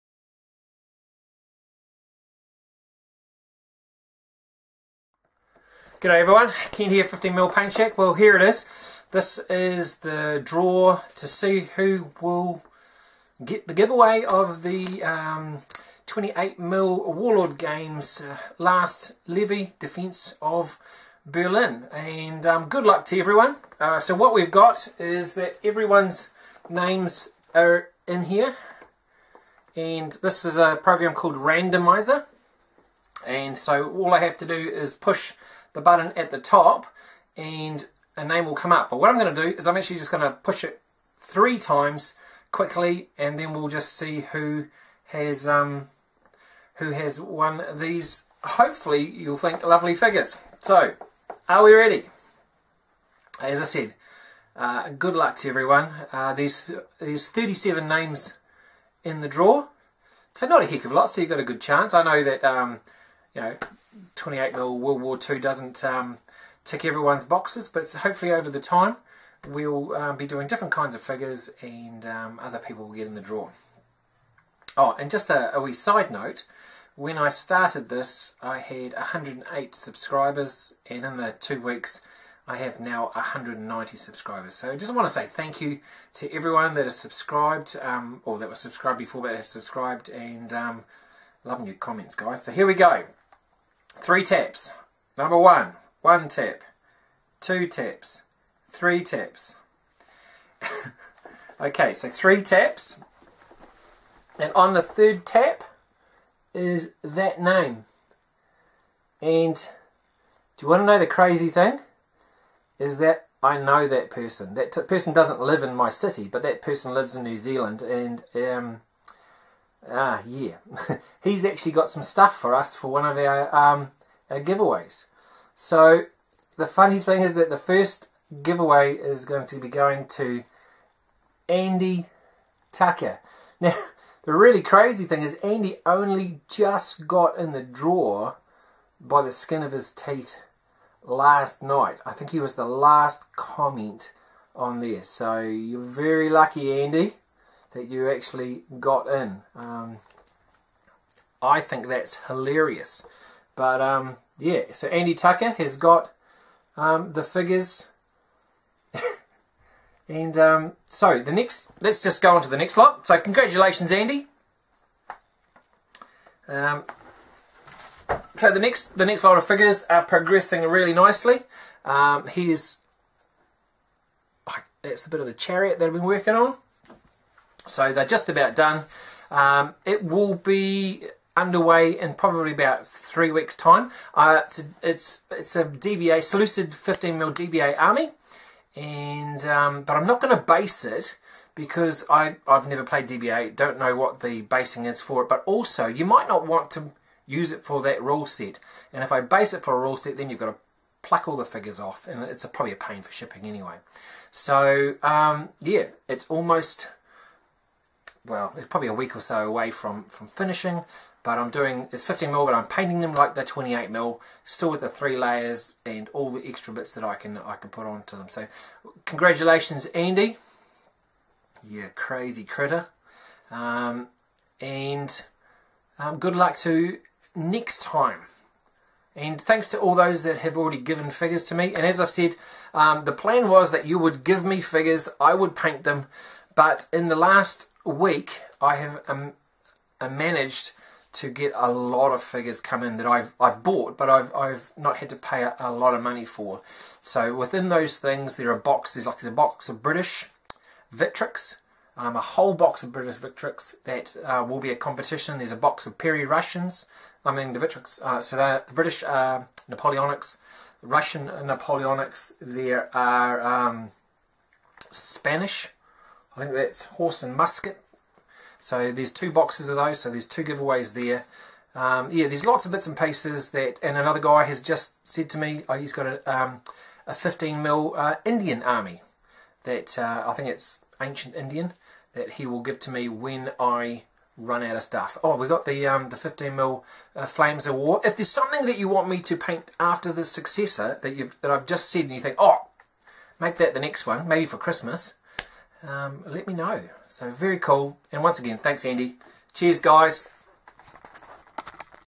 Time to see who wins the 28mm Last levy Defence of Berlin
This video is the draw for the give away Painted boxset